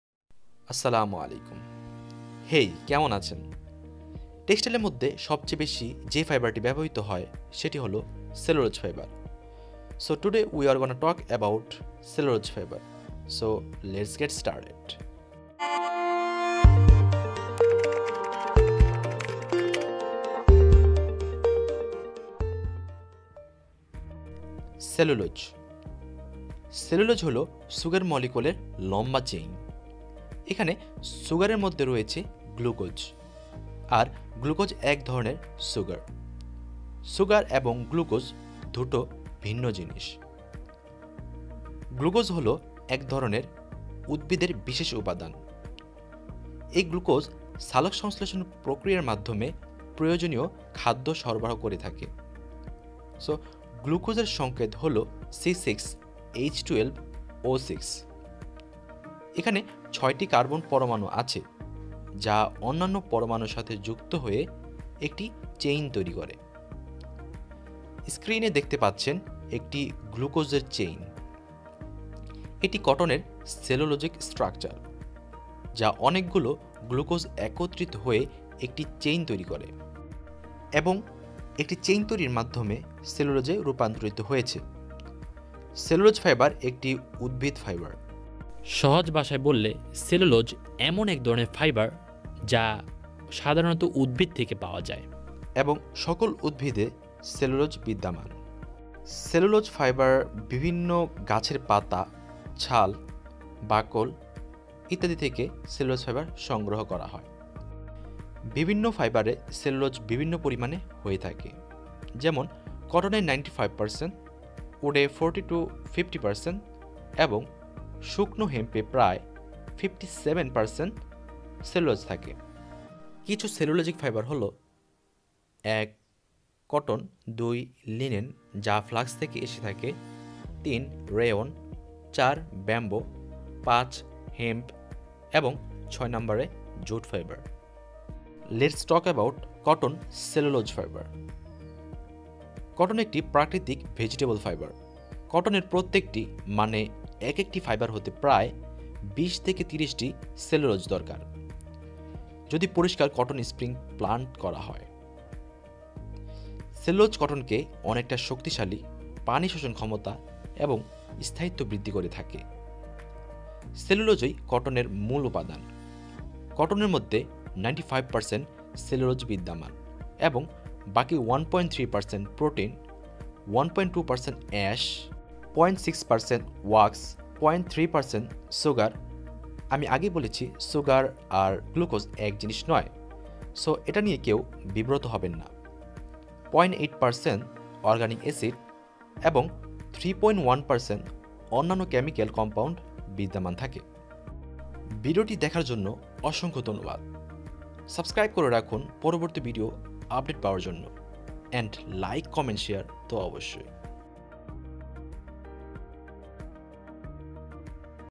Cellulose Fibre (Bangla) | What is Cotton cellulose fibre | Learn Textile
You will get to Know.

What is Cellulose Fibre?
What is Cotton Cellulose Fibre?
Defferences type Cellulose Fibre
Cellulose Fibre Structure
Cellulose Fibres Molecule
Cotton Fibre Constituent Ratio
What does mean by cellulose?
Why cellulose is important?